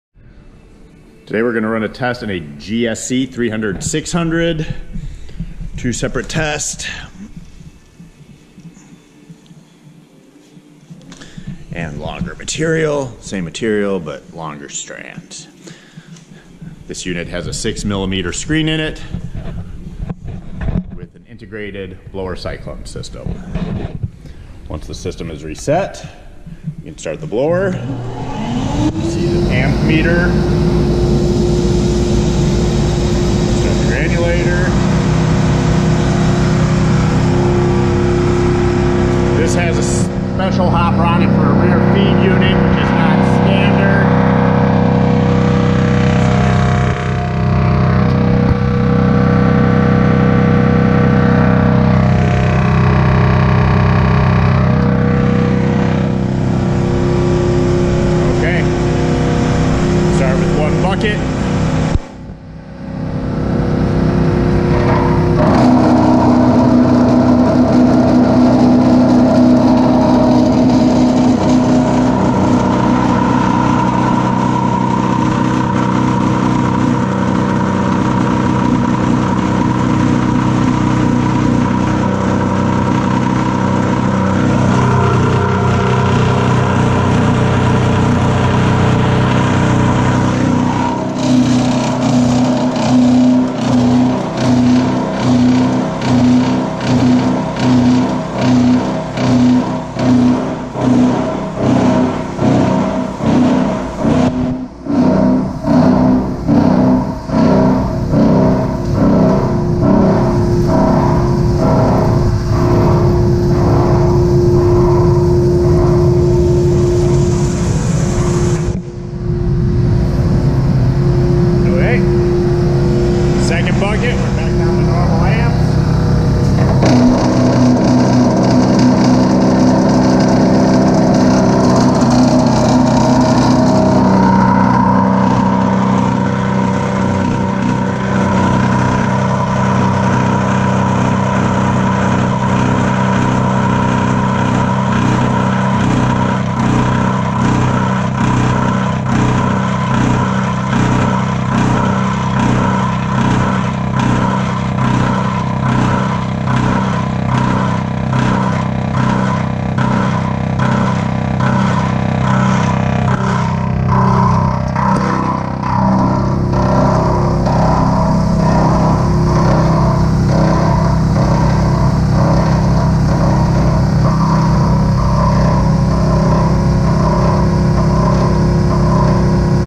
GSC 300 600 Short Fiber 6mm Screen
The GSC compact granulators are designed with a completely soundproof enclosure resulting in an extremely quiet operation. Different rotor designs are available in widths ranging from 12” to 55” (300 mm to 1000 mm). The completely welded cutting chamber in conjunction with the “V” type rotor design ensures dependability in operation and universal application use. While it delivers excellent sound proofing capabilities, it still offers easy and quick access to the cutting chamber during rotor and stator knife changes, servicing or screen changes. The sound dampening material used in these machines is based on the latest technological research.

The cutting geometry of the GSC series allows even voluminous materials to be ground.  The complete series achieves a high-quality regrind independent of the material type or form. The GSC 300 series machines have a small footprint and easy movability make the machines very easy to place in existing operations.